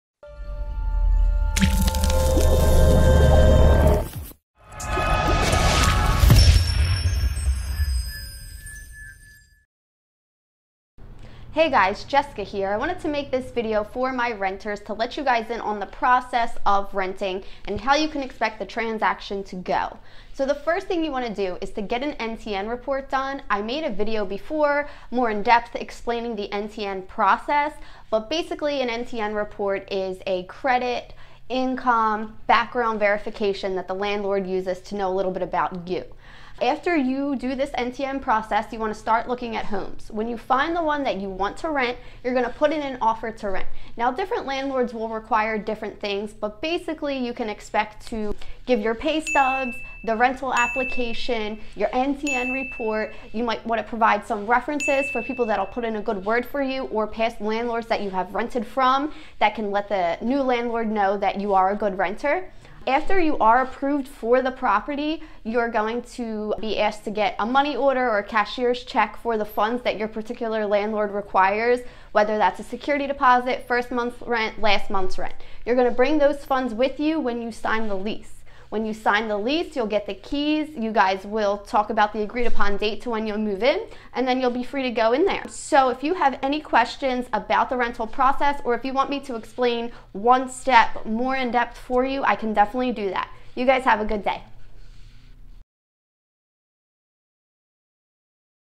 Home Rental Process Explained - How to Rent a Home
Jessica of the FC Real Estate Team with Berkshire Hathaway Home Services explains the process to rent a home in South Jersey.

Berkshire Hathaway Home Services Fox & Roach Mount Laurel
1025 Briggs Road, Suite 148